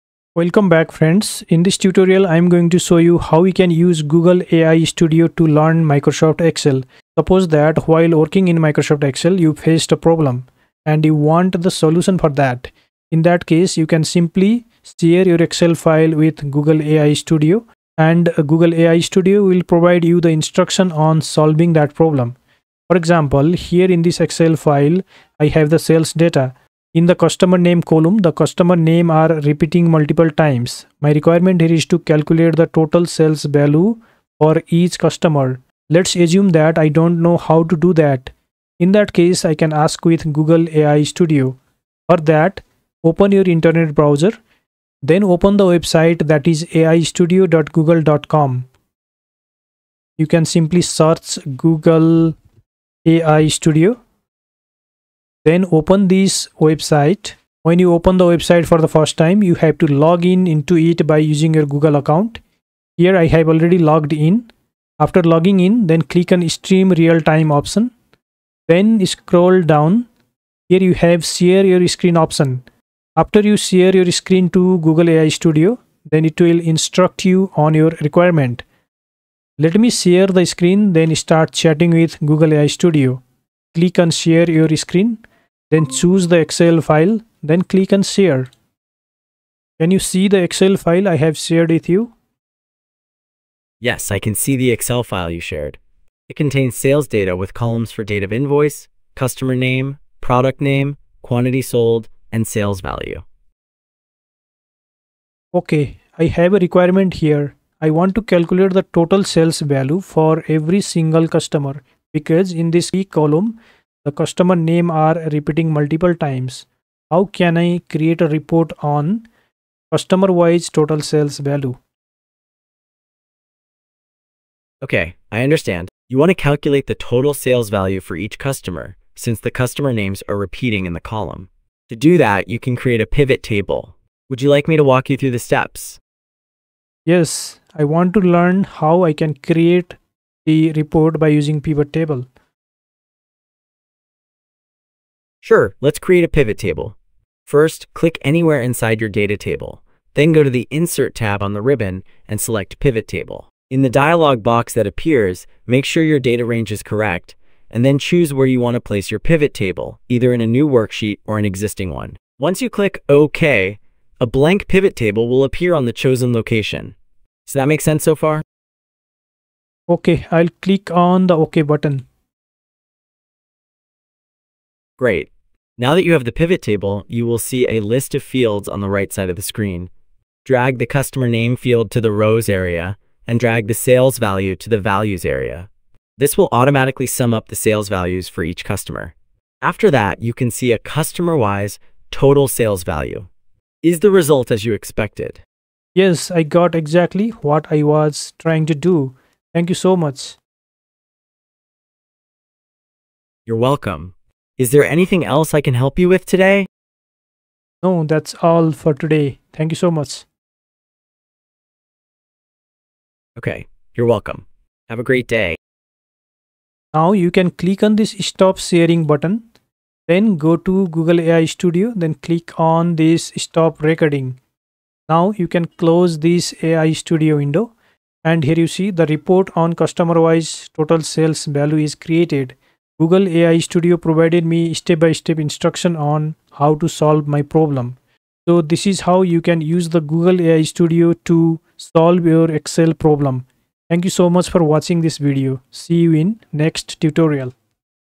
Learn Excel with Google AI Studio | STOP🛑 Watching Tutorials🫢
Learn Excel with Google AI Studio.
In this tutorial you will learn how to learn Excel with Google AI studio.
If you face a problem while working in Excel, you can directly share your Excel screen to Google AI studio and it will provide you the instructions to solve the problem.